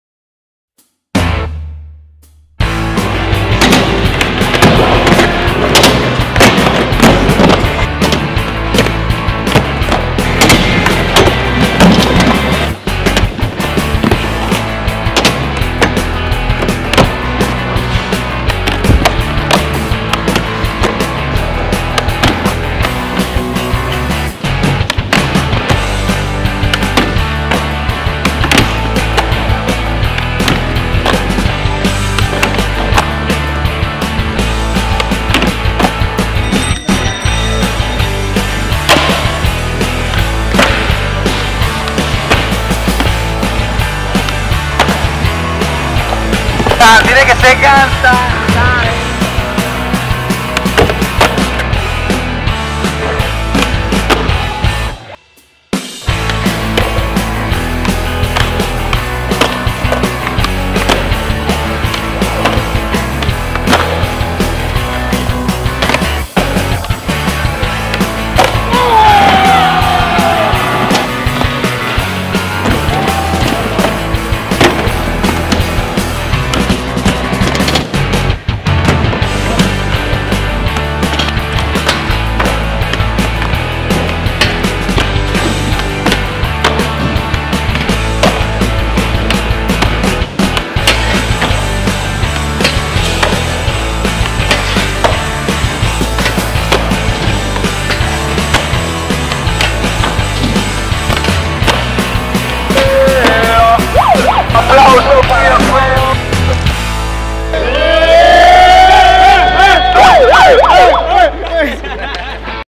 Go Skateboarding Day Panama 2017
Video de lo que fue la celebracion del Go Skateboarding Day en Panama version 2017 en los espacios del skatepark de Paraiso.

Un evento organizado por las marcas Stance, Vans y RVCA

skateboarding panama